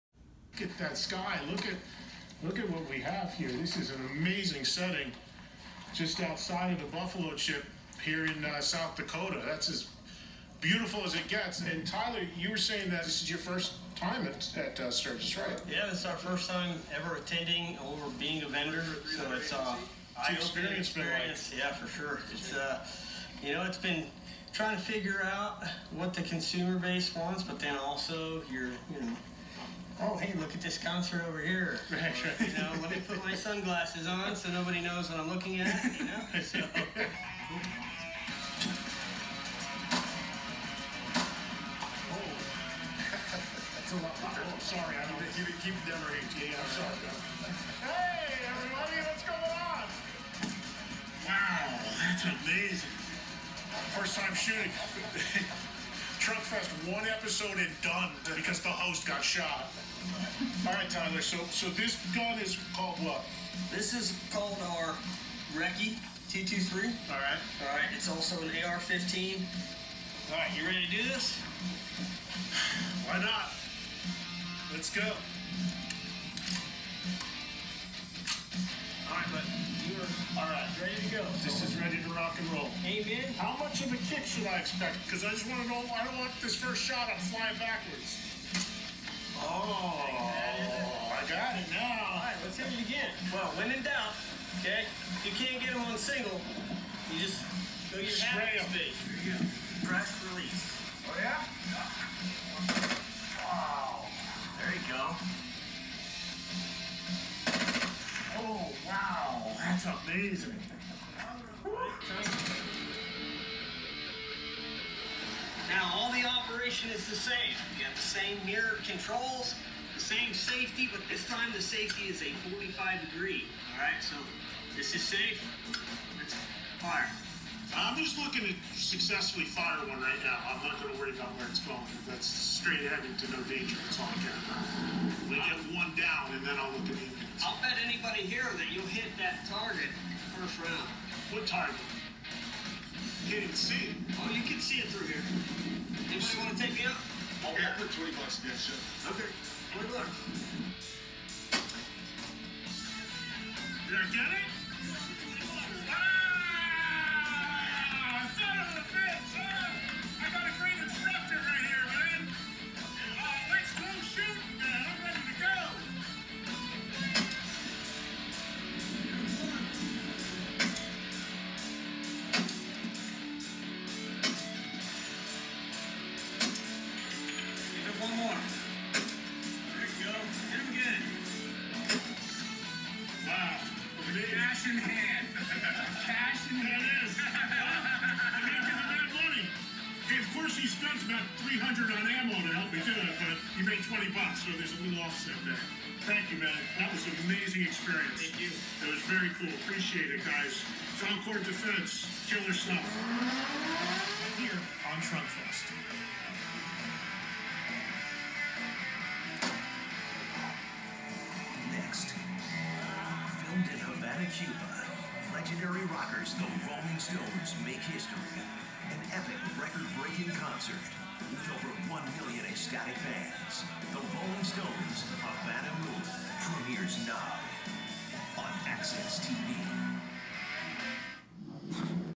TrunkFest: Sturgis Edition
A little range time with Eddie Trunk on his show.